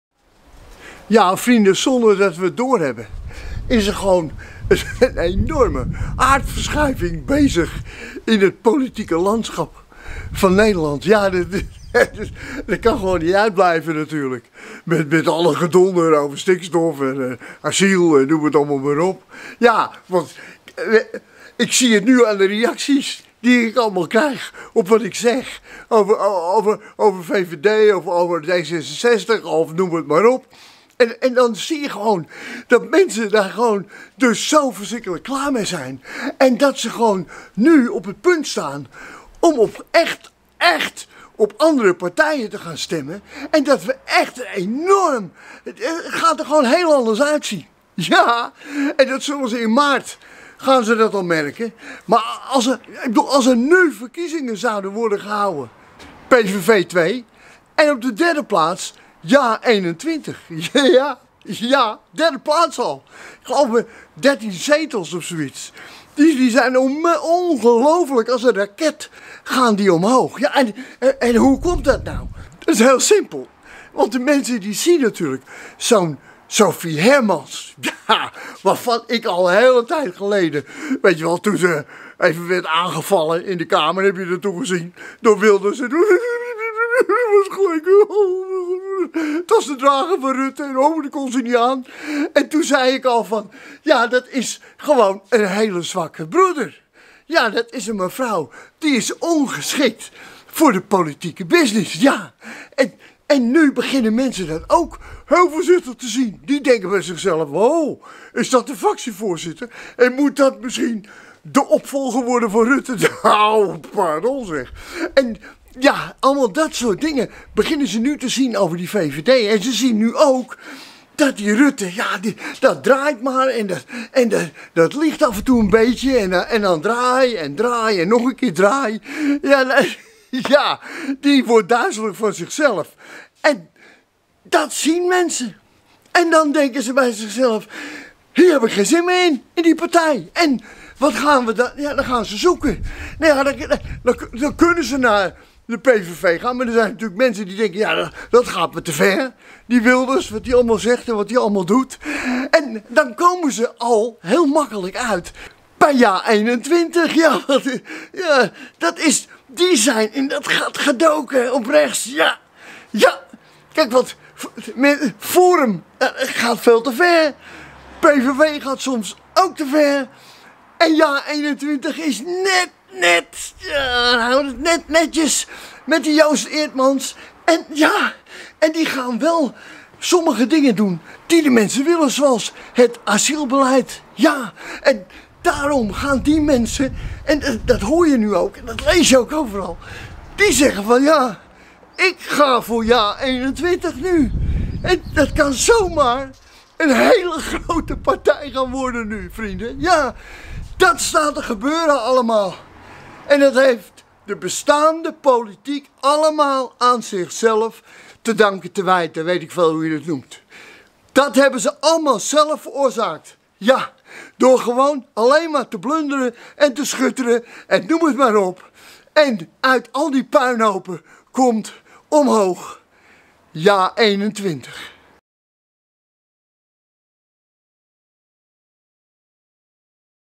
JA21 gaat profiteren van het prutsende kabinet!
JA21 gaat profiteren van het prutsende kabinet! Want als de rook van de puinhoop van deze regering is opgetrokken dan zal je zien dat Eerdmans komt bovendrijven!